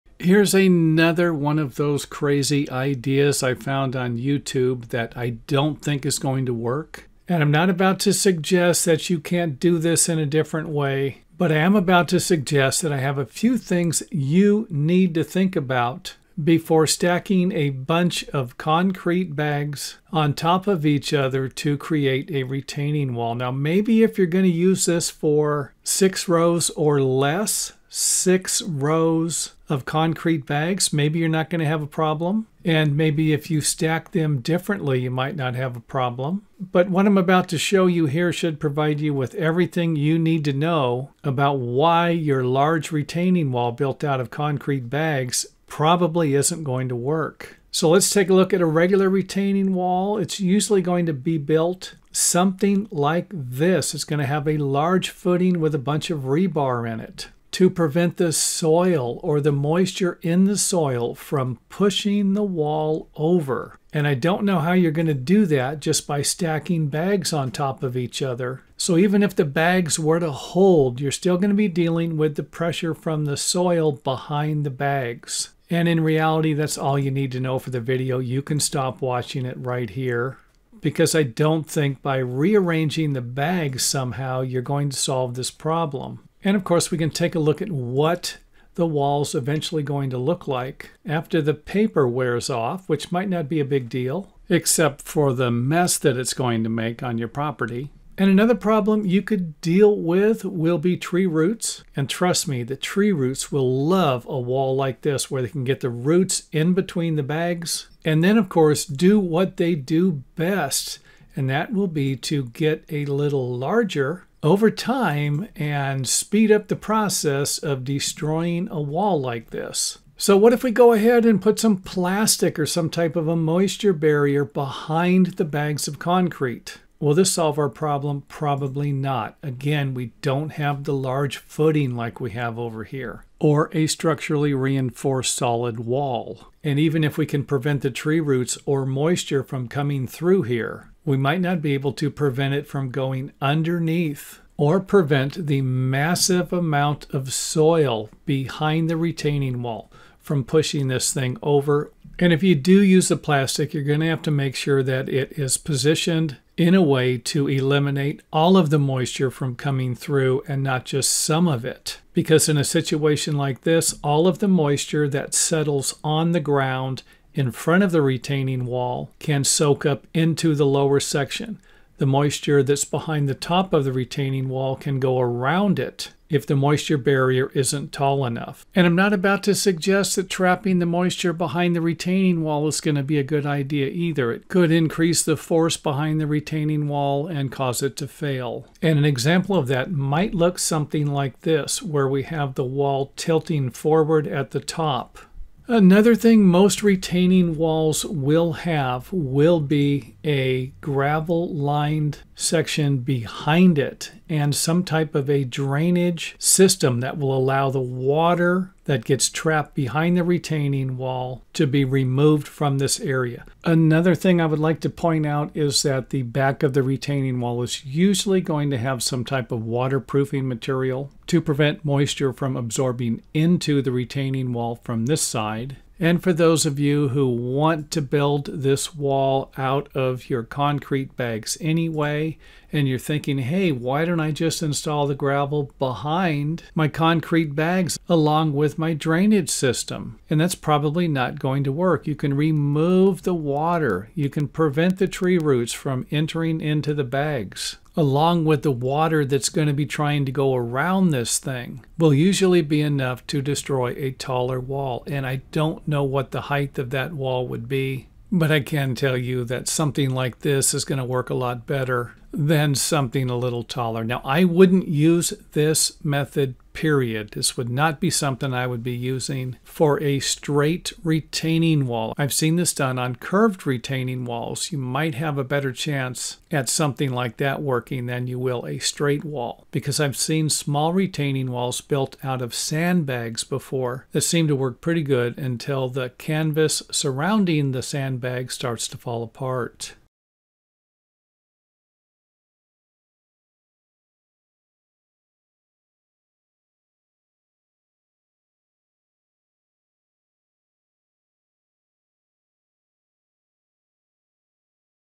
Could These Become Problems When Stacking Dry Mix Concrete Bags To Build Vertical Retaining Walls
Click on link to find more helpful videos on landscaping, home building and repairs. In this video I will explain why simply stacking bags of concrete that have not been mixed to build a large retaining wall might not last. The concrete can fall apart because it was not used properly or mixed and with out a solid footing and wall, could simply fall over. There's a lot of structural engineering requirements that go into designing and building most large retaining walls, which might suggest that it will take a little more than stacking concrete bags in a variety of different positions or driving rebar through them to build a long lasting retaining wall.